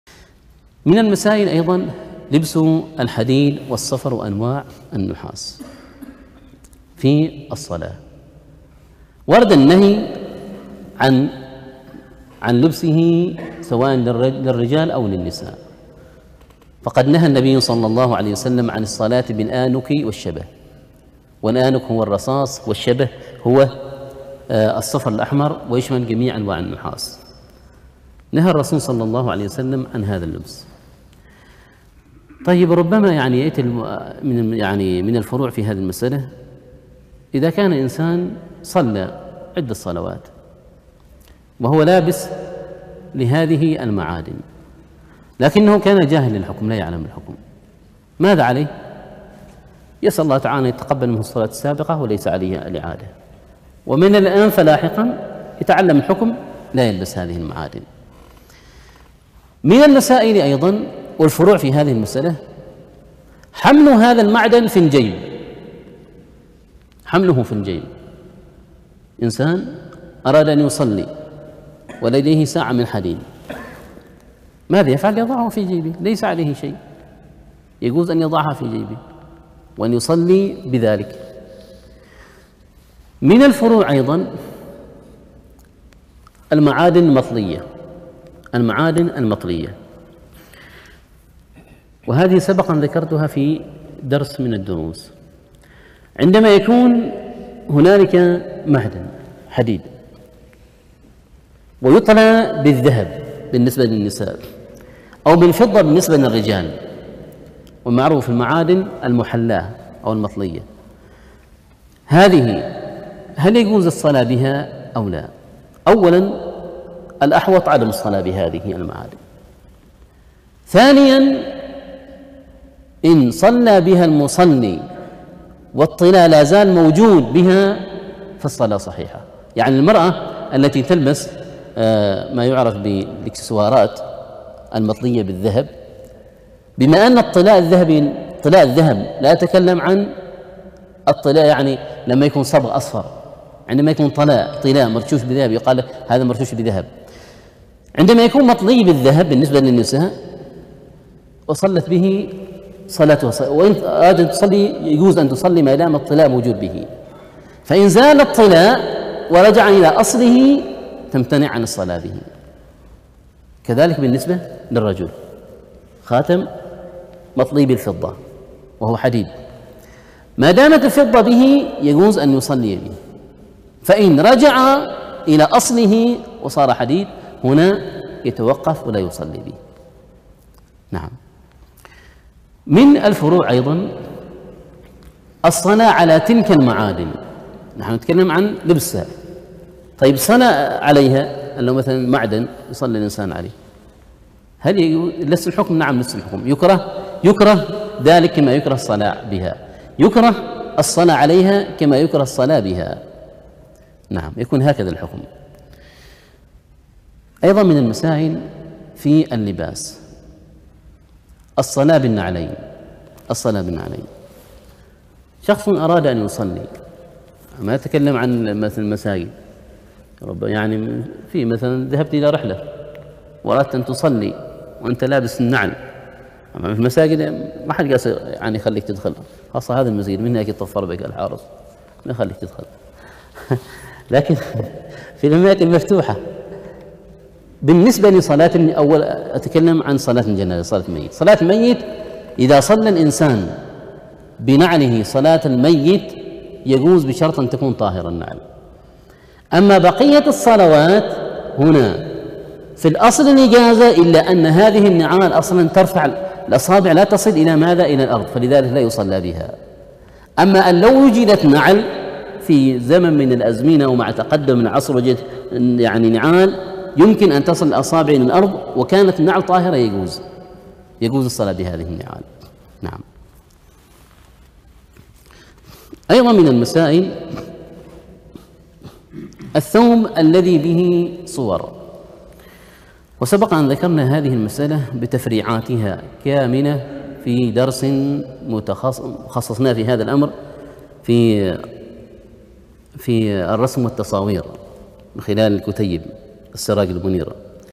مسائل أحكام في لبس الصلاة: لبس الحديد والانك وحمل المعدن والمعادن المطلية. للأستاذ أحمد الحراصي
مصدر المقطع:

محاضرة بعنوان أحكام في لبس الصلاة

للأستاذ أحمد بن حمدان الحراصي